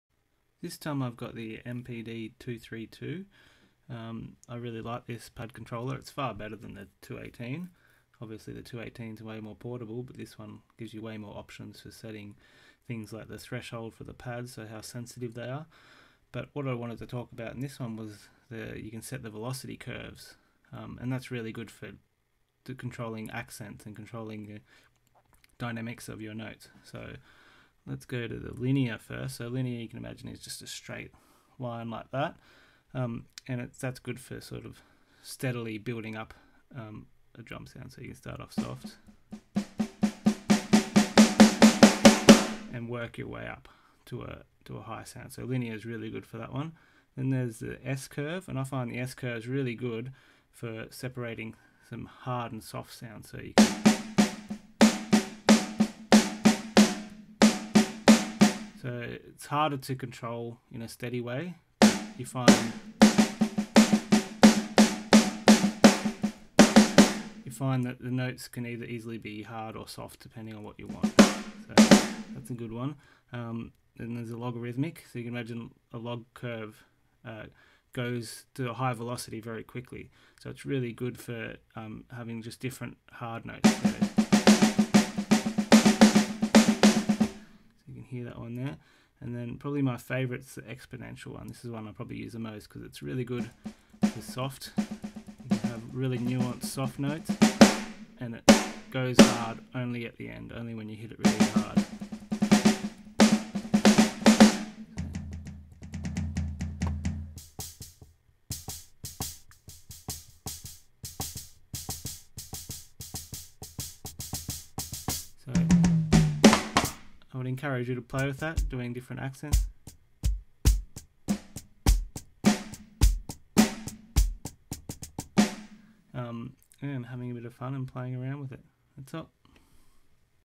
Finger Drumming Lesson 11 Velocity Curves, Dynamics and Accents
Play with velocity curves to achieve different dynamic results